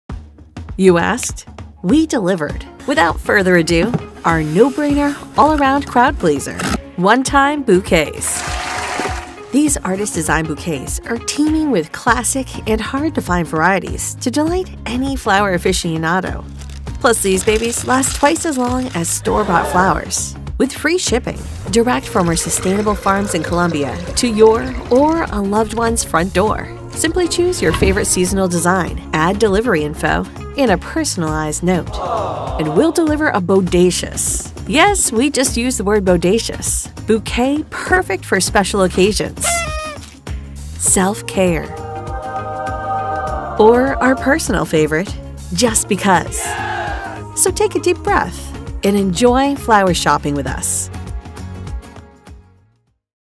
One Time Bouquets Perfect for Every Special Occasion│Enjoy Flowers
You ask and we delivered! Give a warm welcome to the new fan favorite: One Time Bouquets! These seasonally-inspired bouquets are perfect for birthdays, anniversaries, just because, and so much more! Send all the beautiful seasonal blooms from Enjoy Flowers without the commitment.